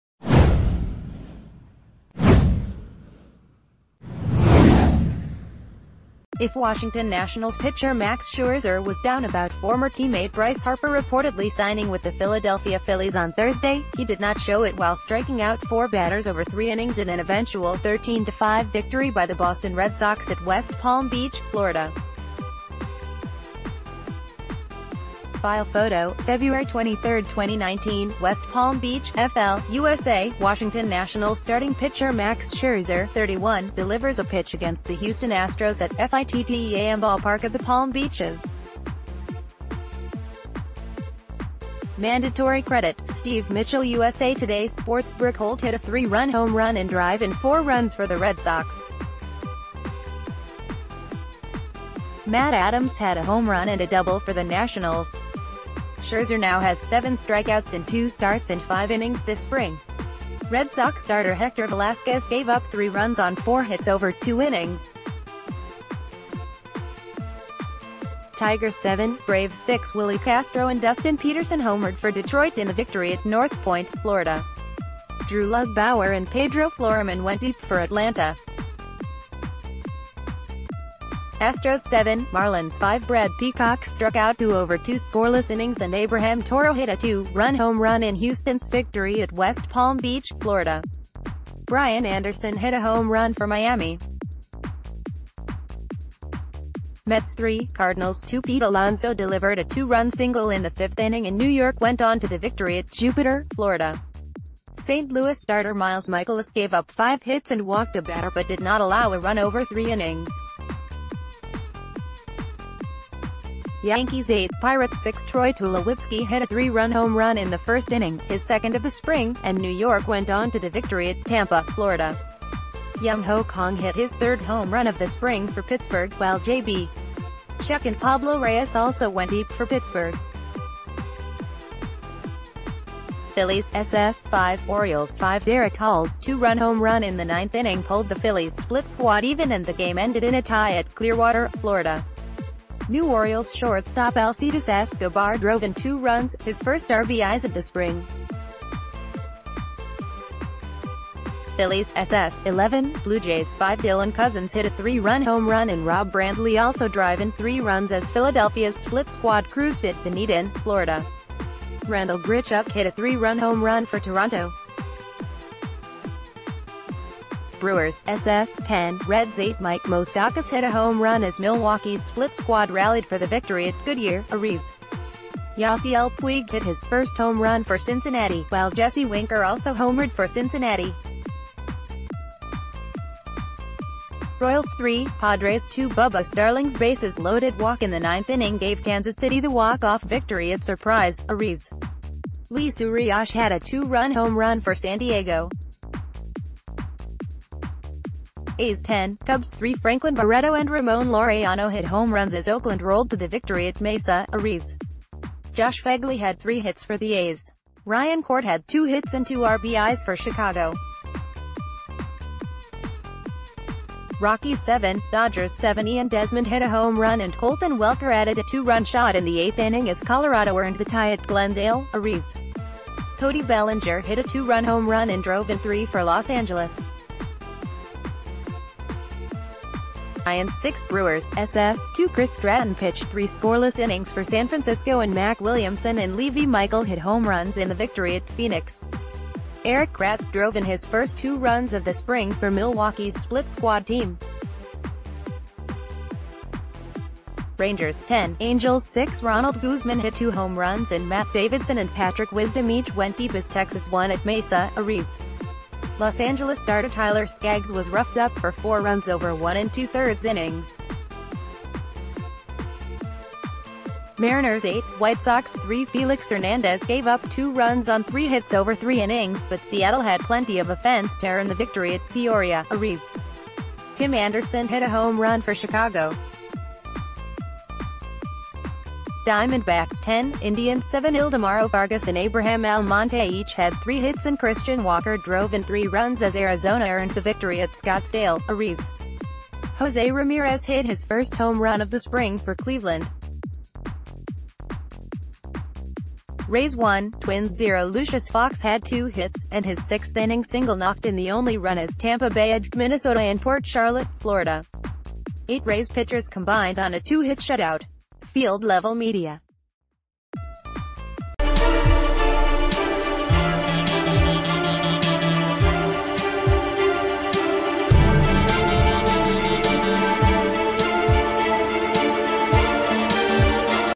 Spring training roundup: Nationals' Scherzer fans four
If Washington Nationals pitcher Max Scherzer was down about former teammate Bryce Harper reportedly signing with the Philadelphia Phillies on Thursday, he did not show it while striking out four batters over three innings in an eventual 13-5 victory by the Boston Red Sox at West Palm Beach, Fla. FILE PHOTO - Feb 23, 2019; West Palm Beach, FL, USA; Washington Nationals starting pitcher Max Scherzer (31) delivers a pitch against the Houston Astros at FITTEAM Ballpark of the Palm Beaches. Mandatory...